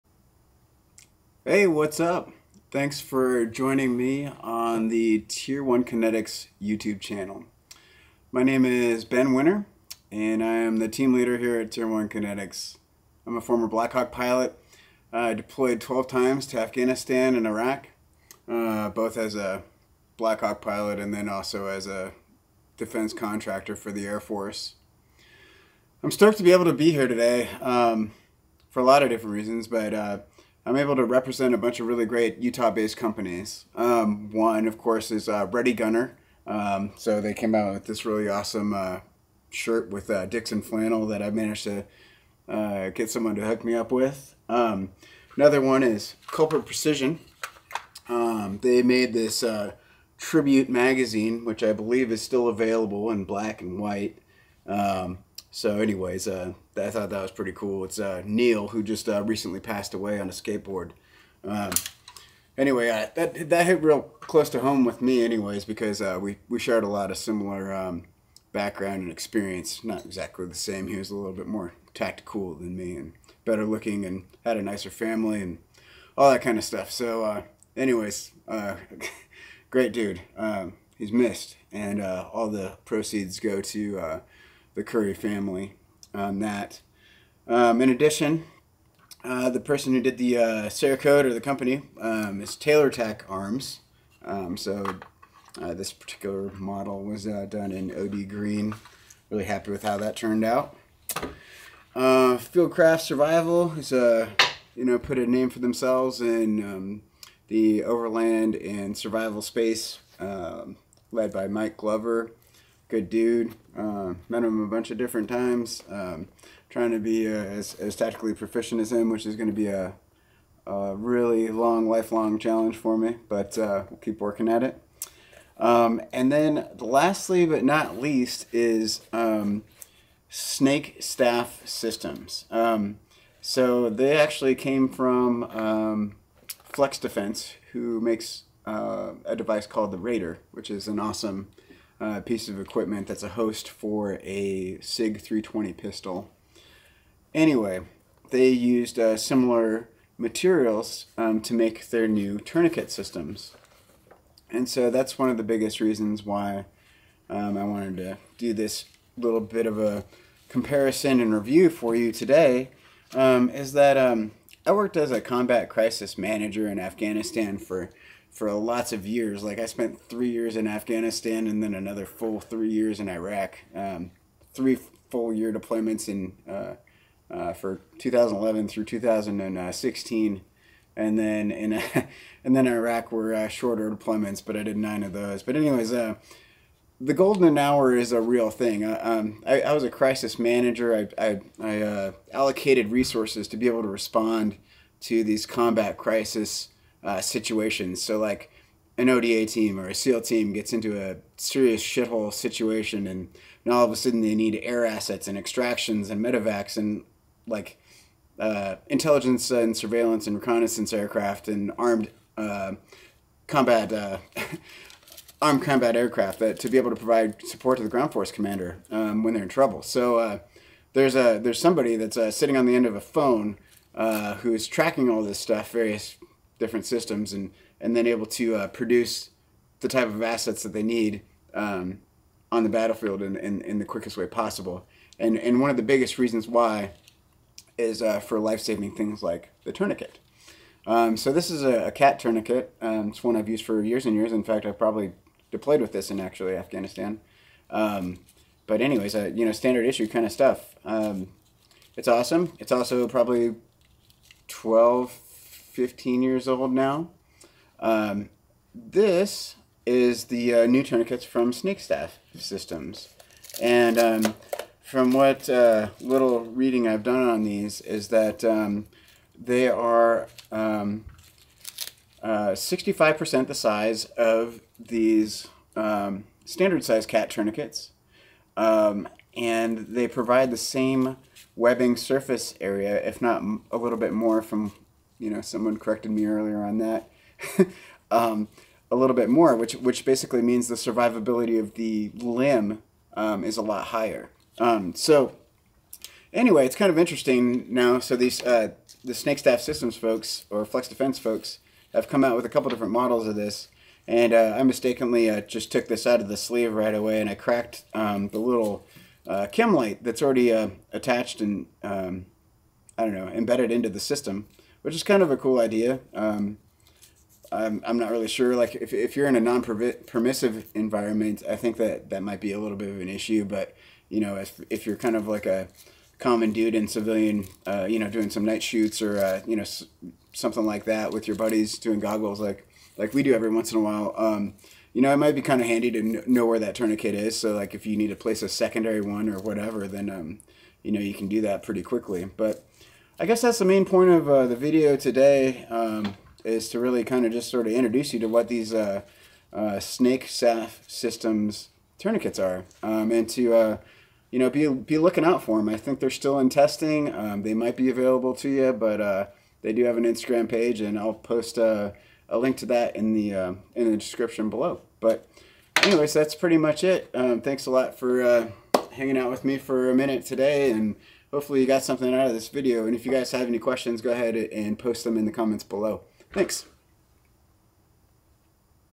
NEW TOURNIQUET BY SNAKESTAFF SYSTEMS
NOTE: I did learn after this video was uploaded that the TQ does come with IR and also visible green chemlights

First impressions of new tourniquet design from SNAKESTAFF Systems